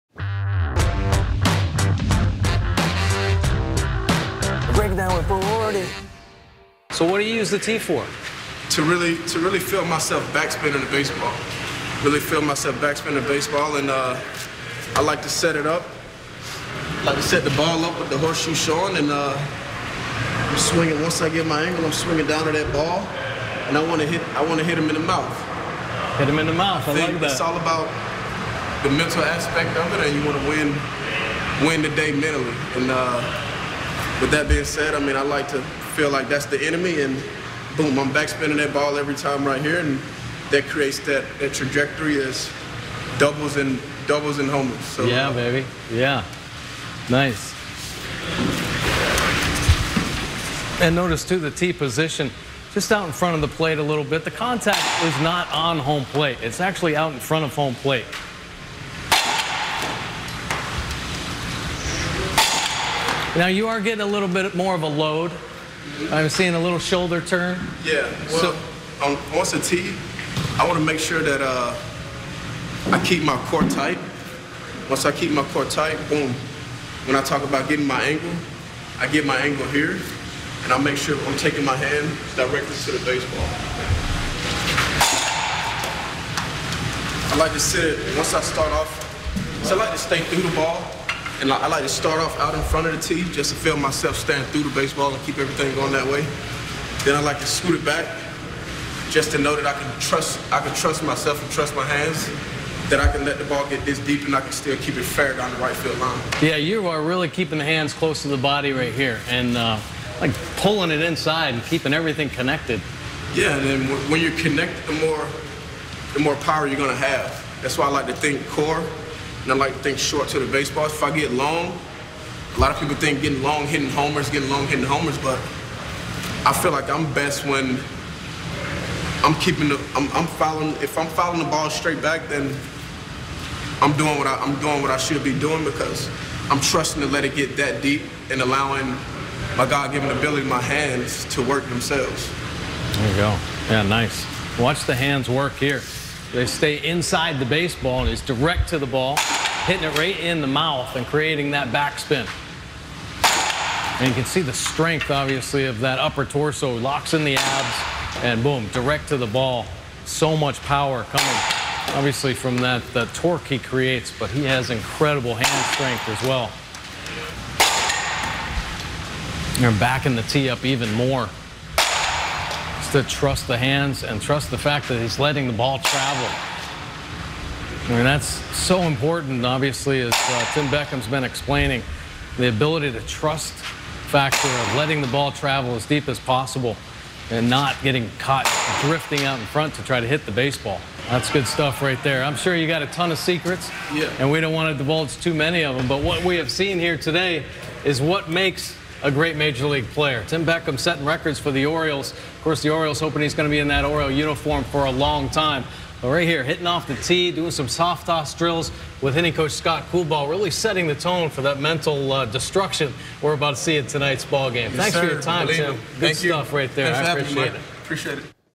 Breaking It Down with Bordick: Tim Beckham on his mentality
Tim Beckham joins Mike Bordick to talk about his mentality and approach to attacking the ball. More